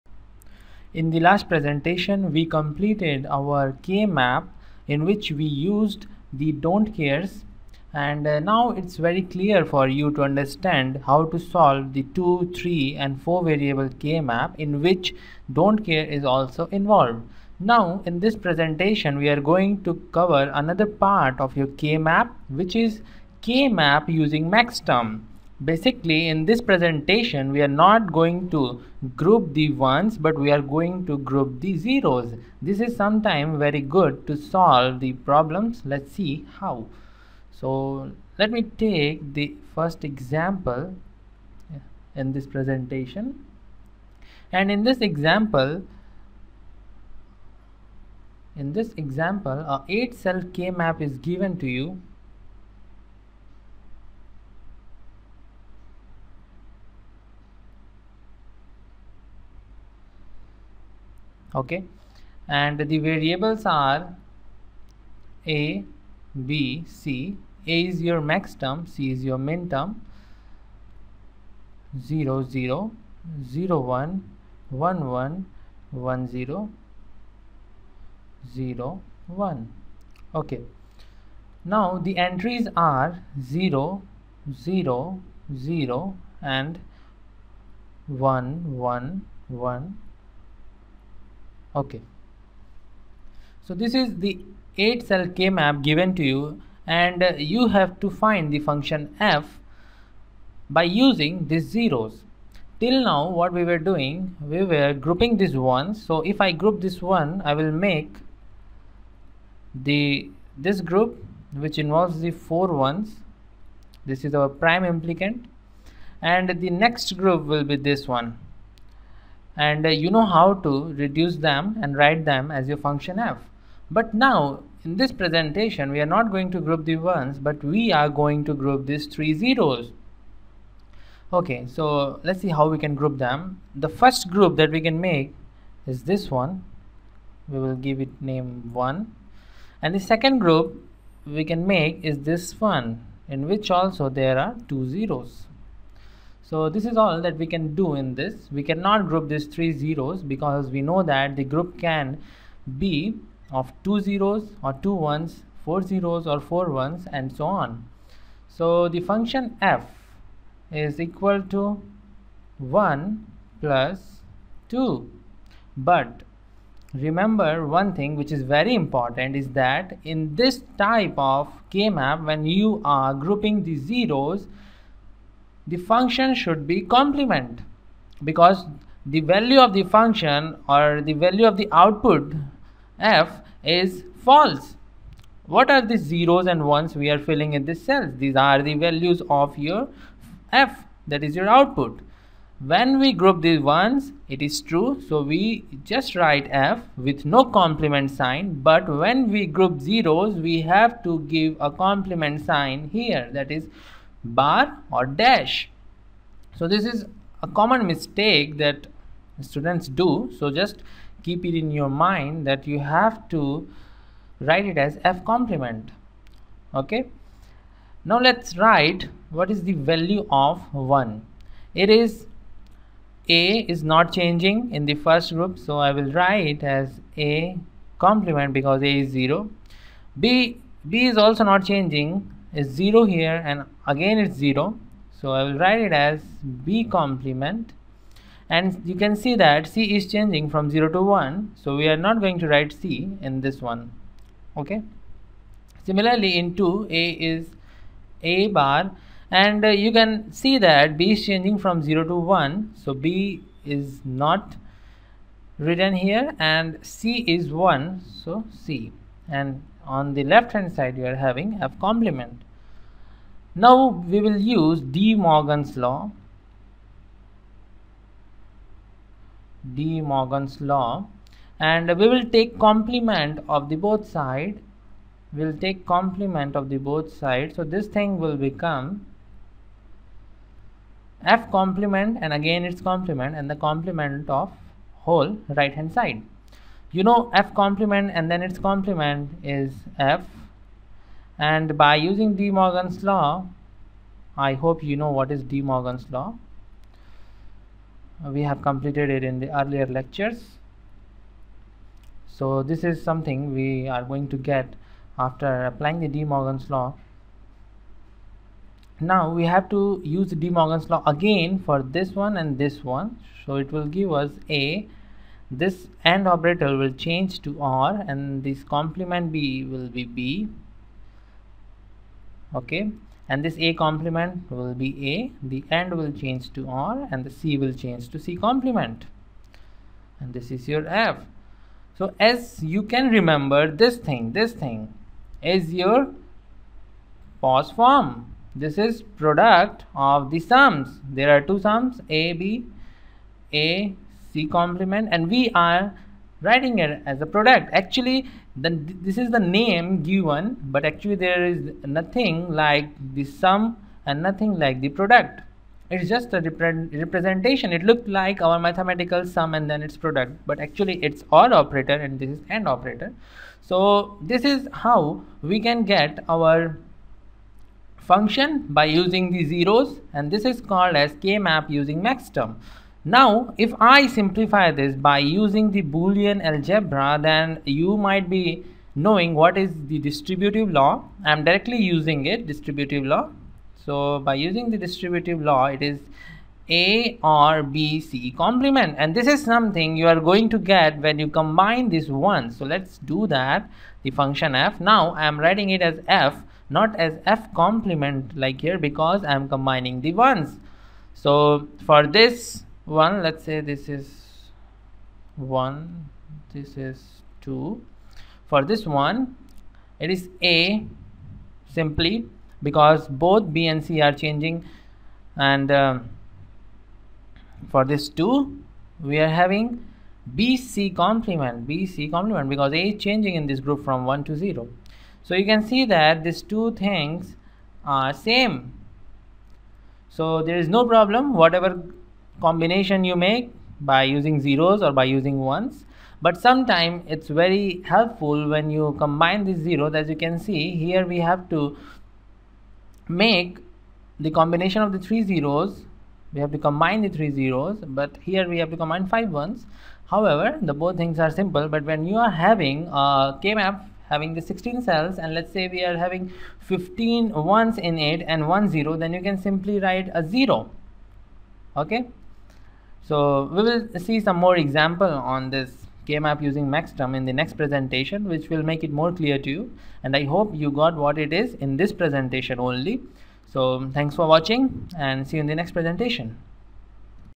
K' Map using Max Terms (Part 1)
Digital Electronics: K' Map using Max Terms (Part 1)

Topics discussed:
1) Introduction to solving K' Map using Max terms.
2) Example on minimizing K' Map using Max terms.